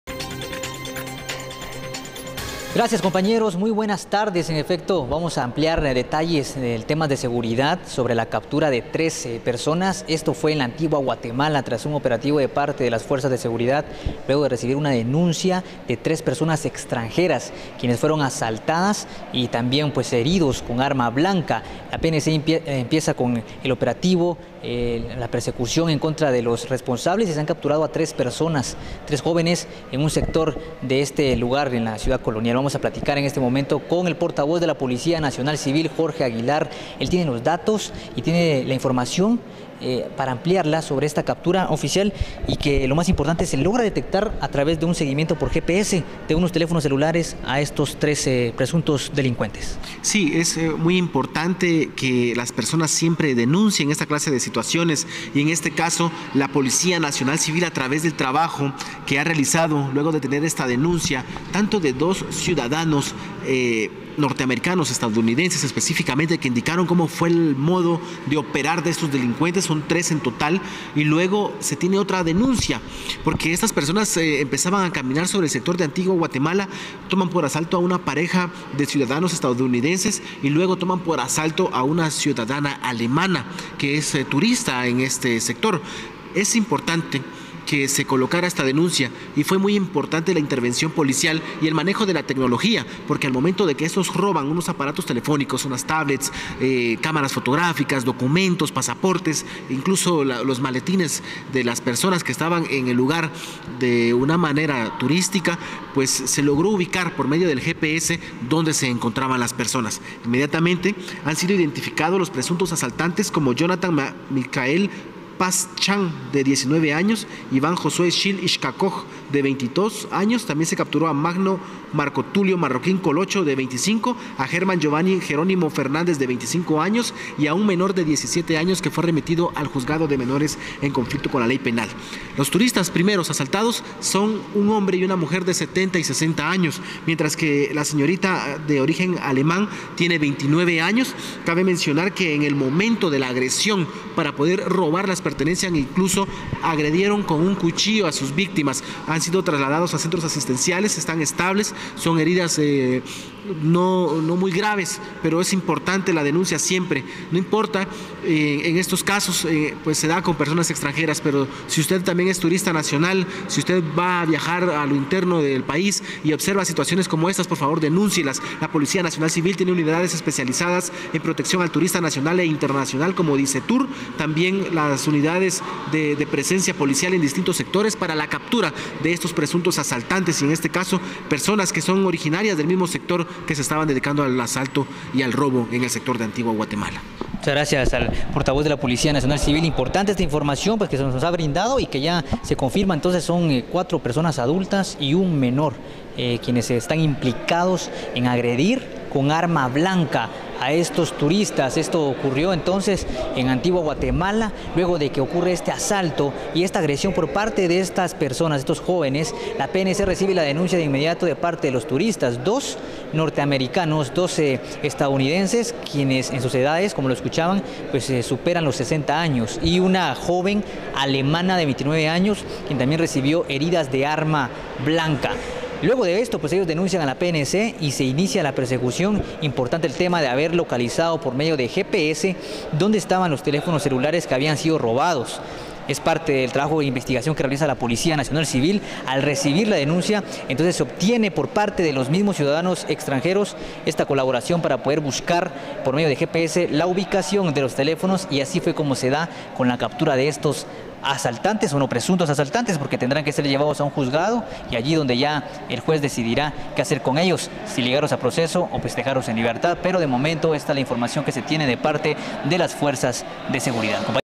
Asaltantes: los espantaturistas
La Antigua Guatemala, es uno de los mayores puntos turísticos, sin embargo, asaltantes también están acudiendo, para despojar de sus pertenencias a turistas, lo cual pone en riesgo el flujo de visitantes en la zona.

Capturan a tres asaltantes en la Antigua Guatemala